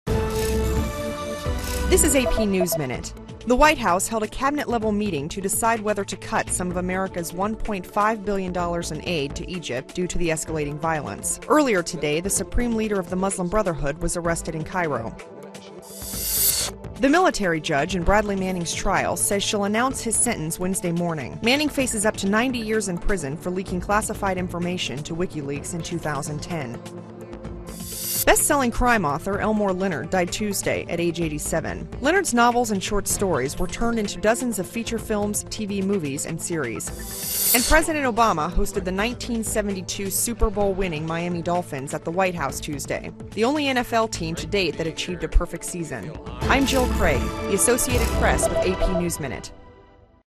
AP Top Stories August 20 P
Here's the latest news for Tuesday, August 20th: US deciding whether to cut aid to Egypt; Manning sentence due Wednesday; Best-selling crime author Elmore Leonard dies at 87; Miami Dolphins honored at White House